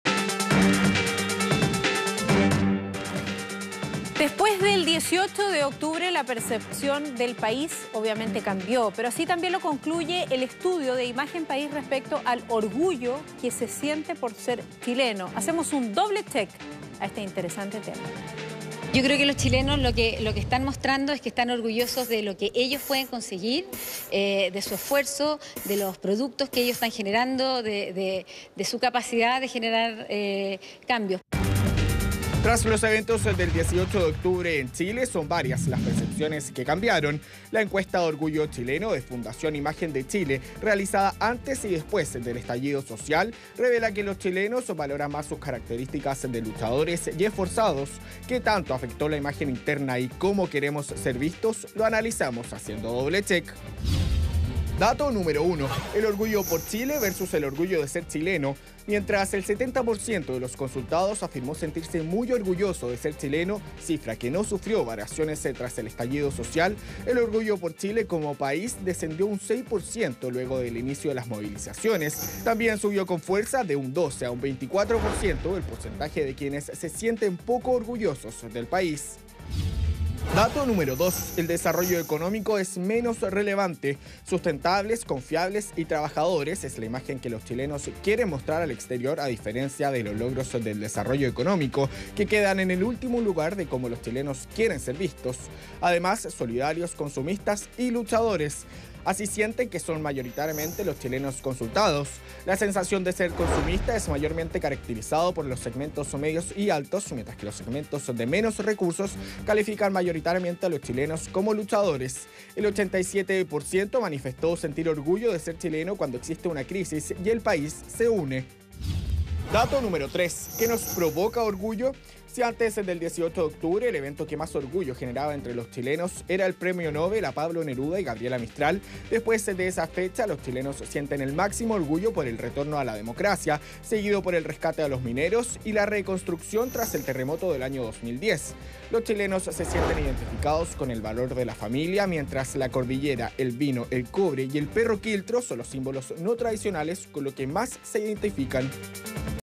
Chilenos cambian su percepción del país tras el estallido social
Después del pasado 18 de octubre, la percepción del país cambió, así lo concluyó el estudio de Imagen País respecto al orgullo que se siente por ser chileno.